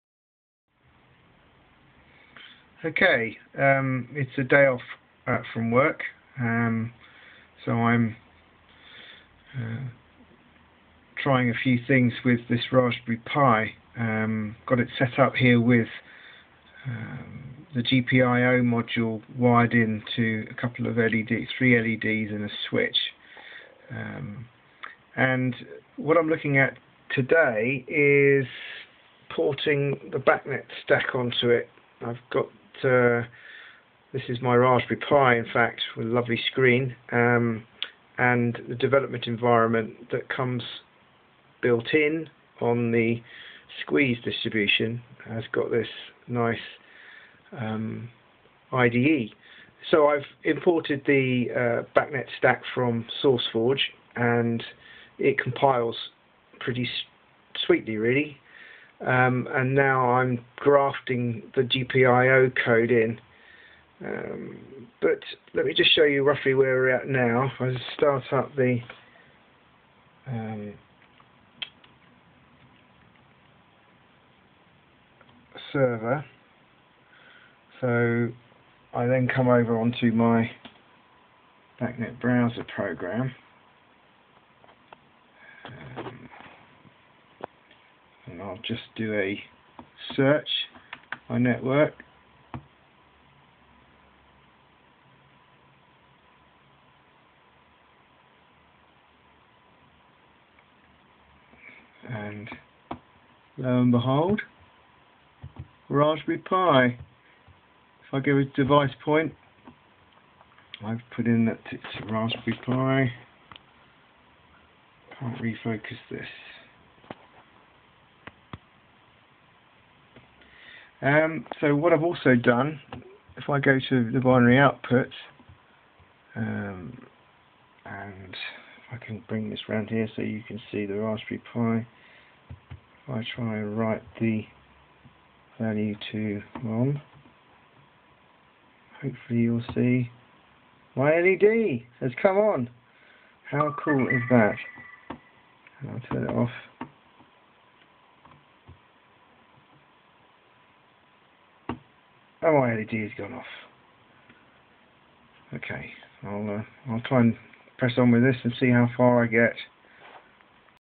Raspberry Pi running BACnet
Very quick demo of BACnet/IP running on a Raspberry Pi.  BACnet is one of the most commonly used communications protocols in building control systems.